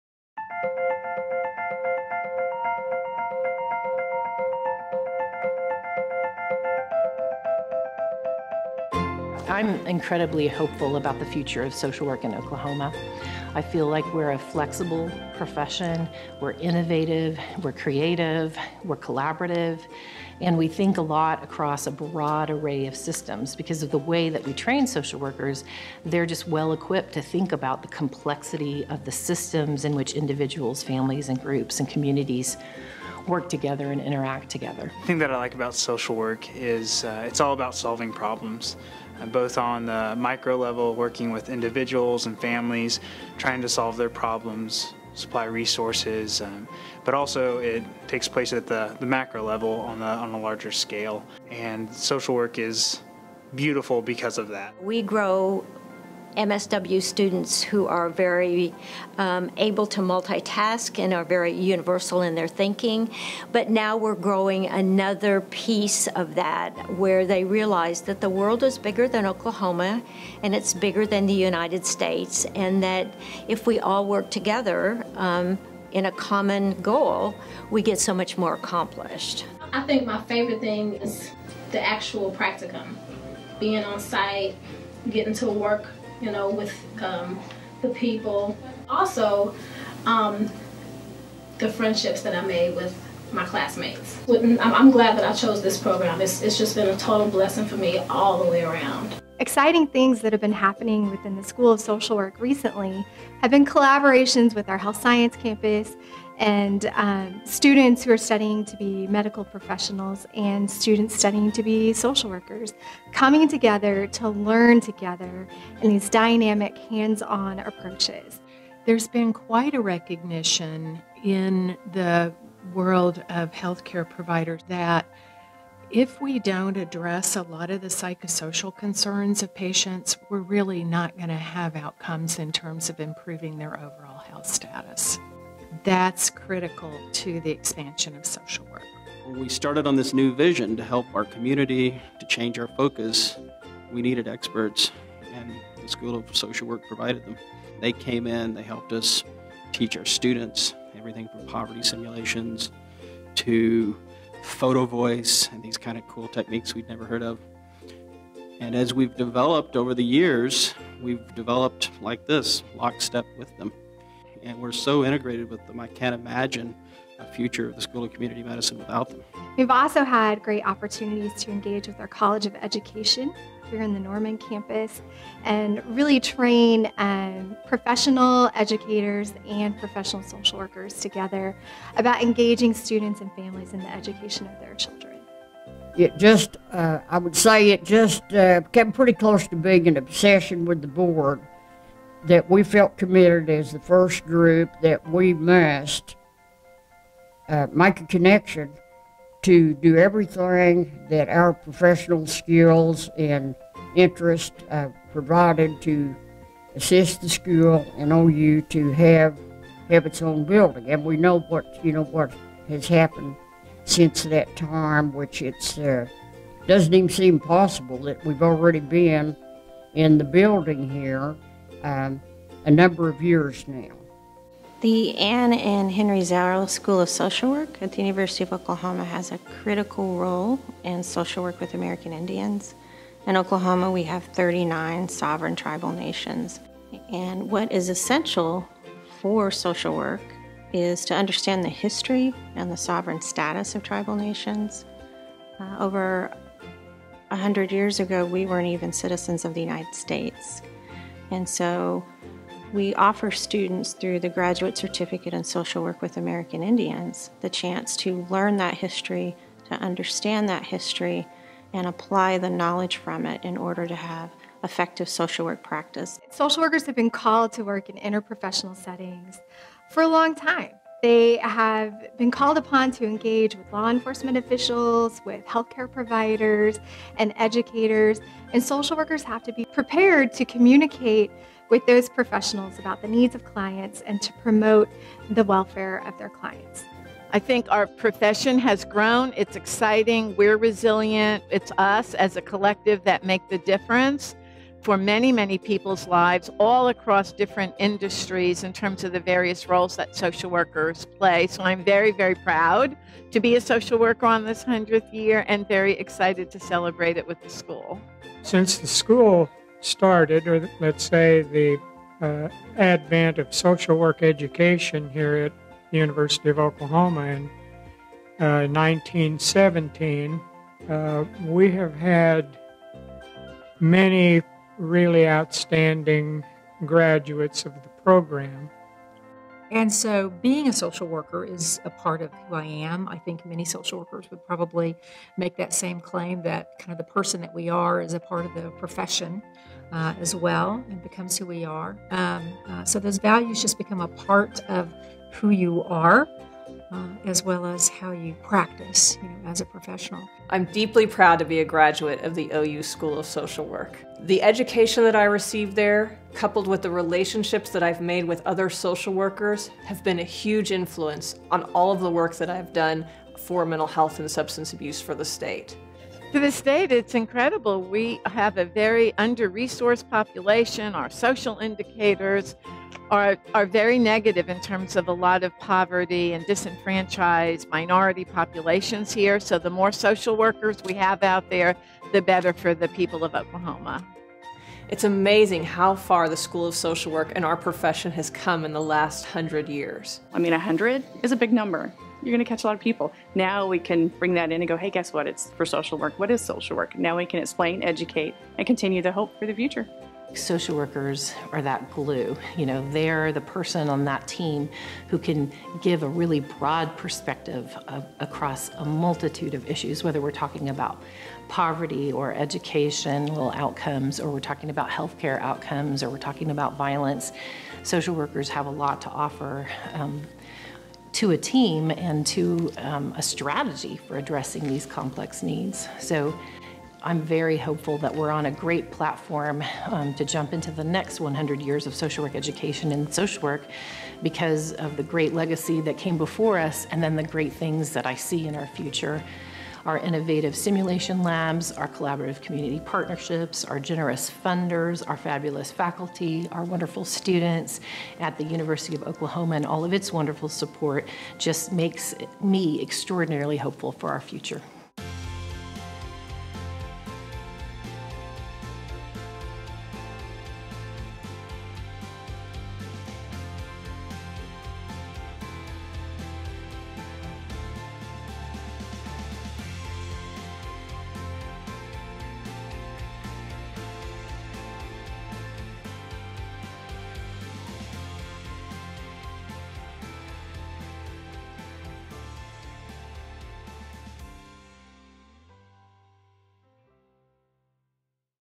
100 Years of Social Work
Celebration of the 100th anniversary of the University of Oklahoma Anne & Henry Zarrow School of Social Work